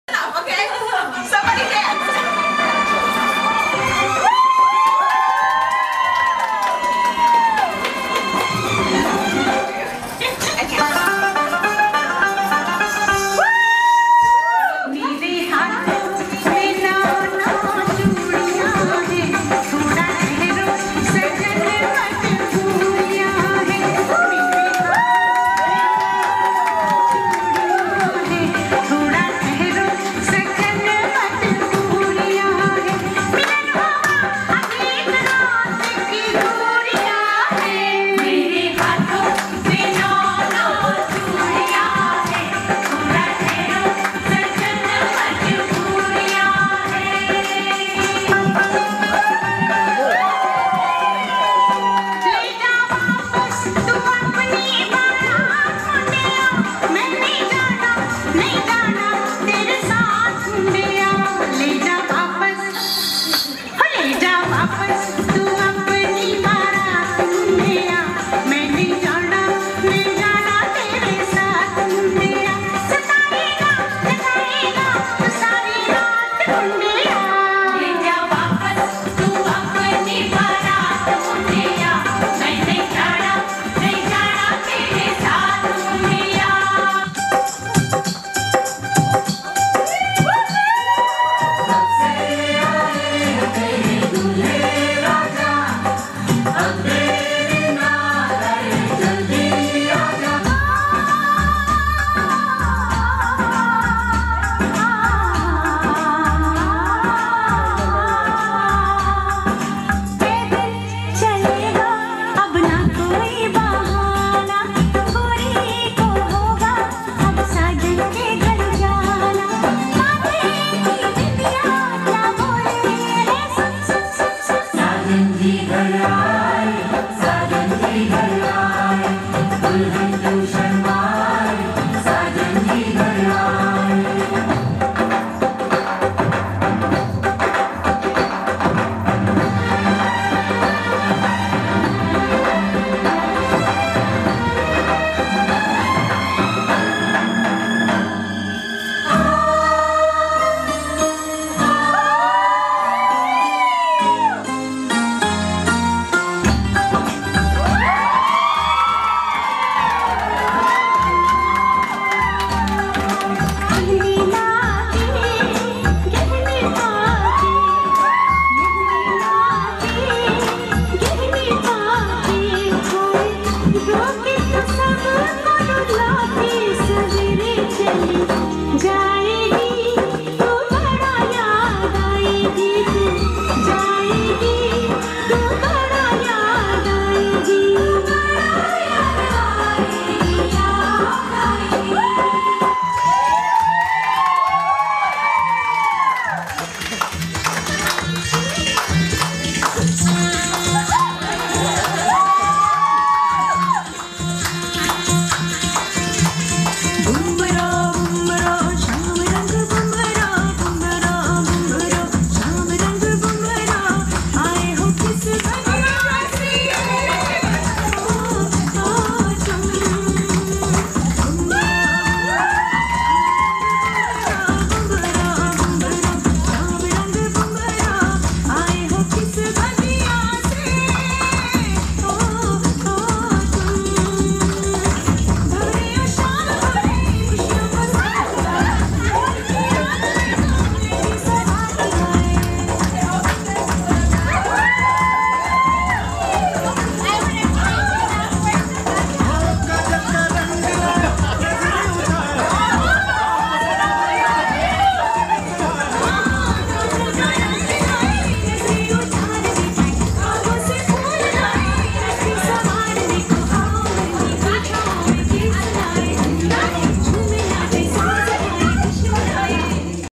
Elisha's mehendi night dance
Elisha's mhendi night dance proformance.
Dance by Akshata Singh
Videos by Lori Thantos and Neekil Singh